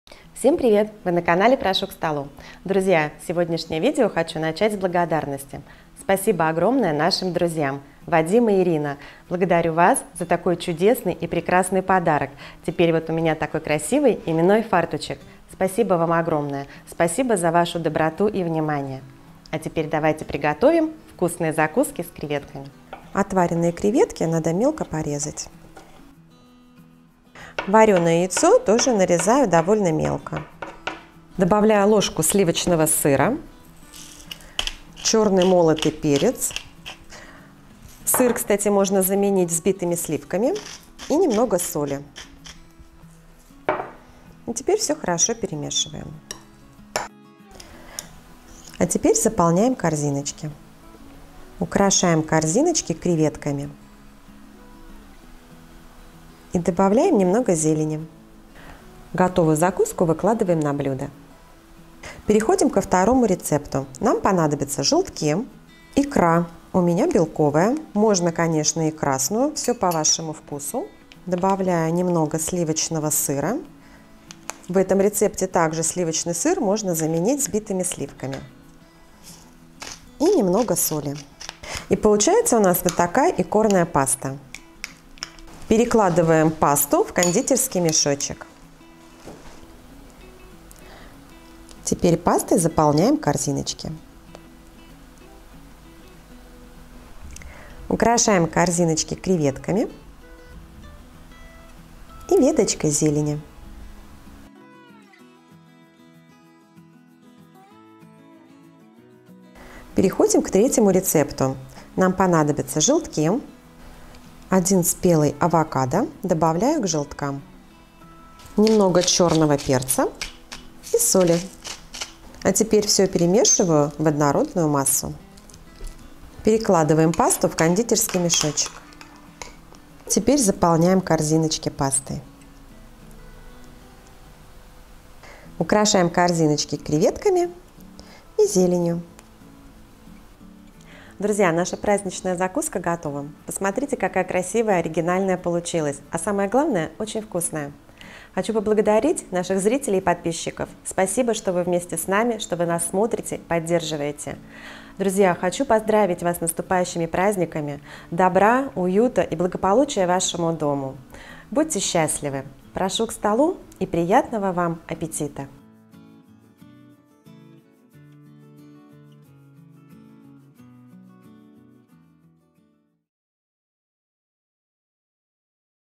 ЗАКУСКИ 2022! Вкусные Красивые Простые Закуски с Креветками. Новогоднее меню 2022.
Супер Вкусные Закуски с Креветками!

Рецепты:
1.Креветки
   яйца
   корзиночки
   сливочный сыр (можно заменить на взбитые сливки 35%)
   соль, черный перец
2.Креветки
   желтки
   икра белковая или лососевая
   корзиночки
3.Креветки
   авокадо
   желтки
   корзиночки
   соль, черный перец